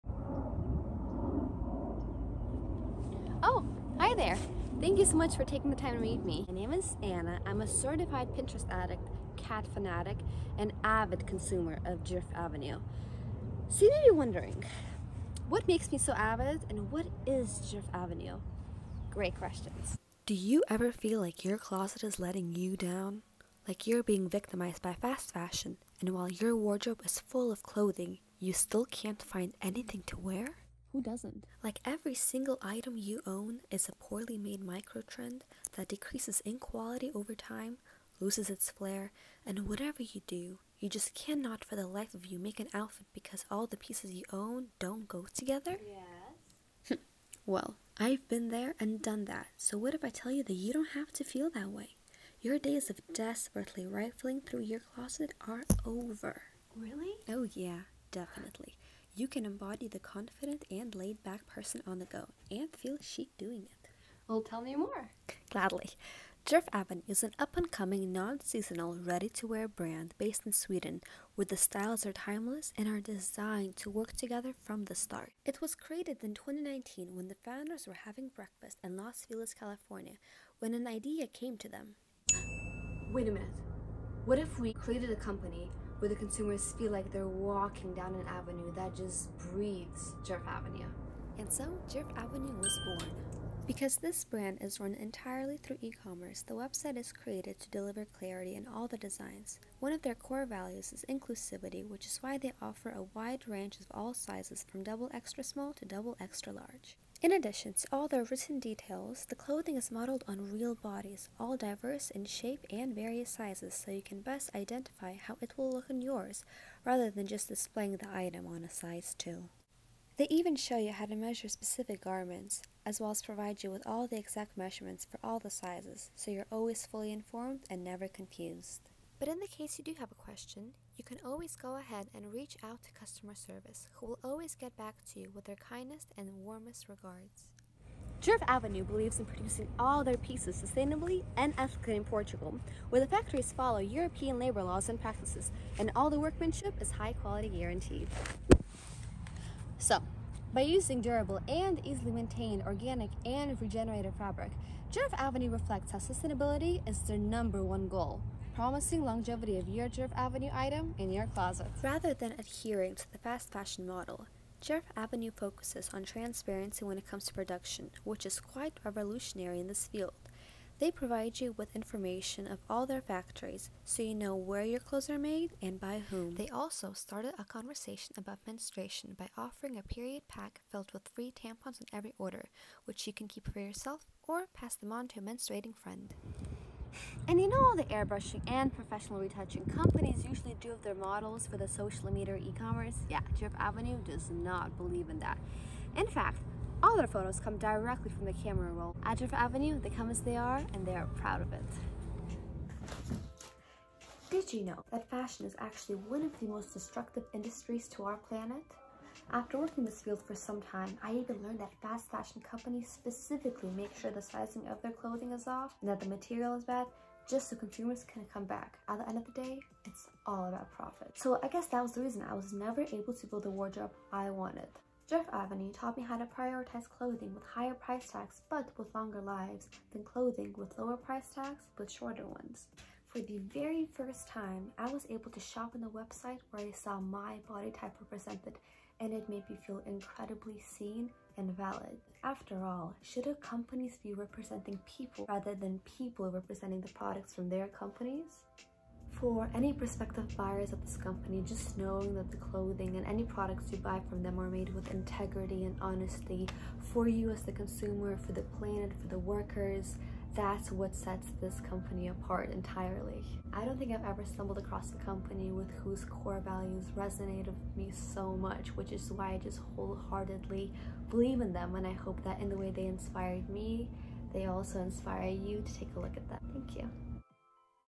Knoji Brand Values Scholarship: Djerf Avenue’s Brand Mission & Values
Why I love Djerf Avenue!